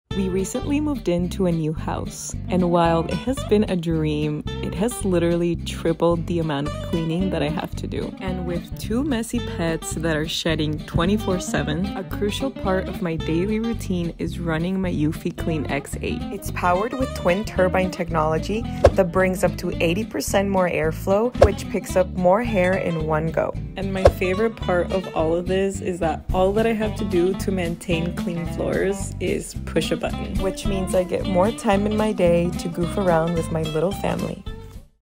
How to manage pet hair in the home! dogs dogmom funnydogs comedy tiktok AD
My favorite robot vacuum, the Eufy Clean X8, is on sale for Black Friday! Now get $200 off when you click this 🛒 (discount applied at checkout)

If your pets shed a lot of hair, this is a must have to maintain a fur-free floor! The suction on this thing is POWERFUL💥 I have been testing it out for 3 months now, and I run it every day.. it just helps me out so much!

eufypartner [AD]